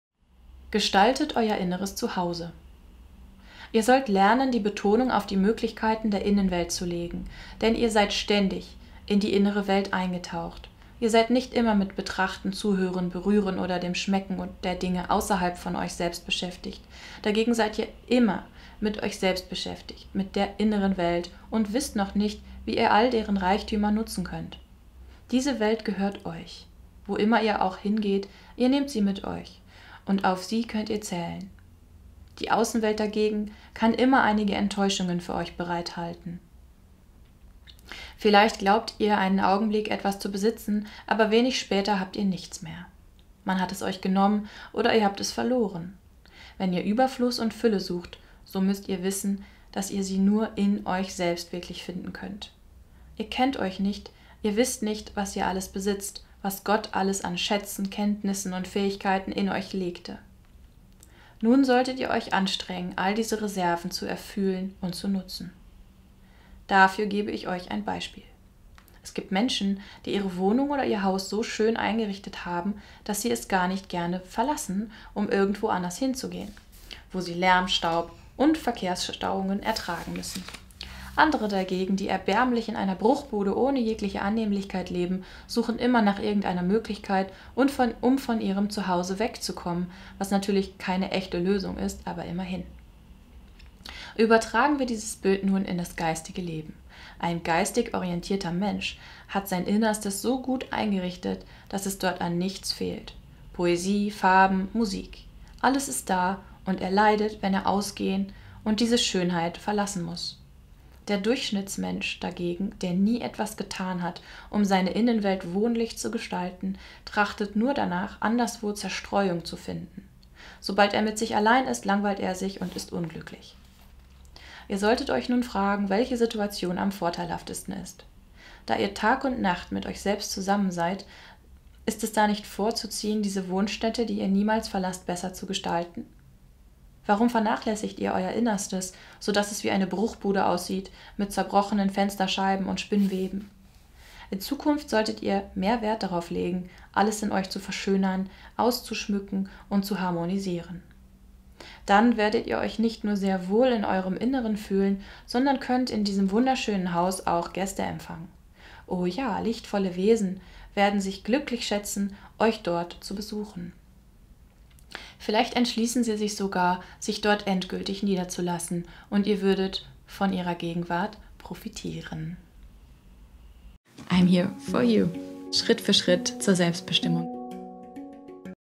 GESTALTE DEIN INNERES ZUHAUSE I Omraam Mikhaël Aïvanhov gelesen von Miri Simsek
Omraam Mikhael Aivanhovs Vorträge sind verschriftlicht Worten in gesammelten Werken sowie diesen kleinen bunten Handbüchern der Reihe Izvor (Verlag Prosveta). Diese Serie umfasst alle Goldenen Regeln des Alltags von #1 bis #117.

Viel Spaß!

Hier kannst du das Buch von Omraam kaufen (Partnerlinks):
Goldene Regeln für den Alltag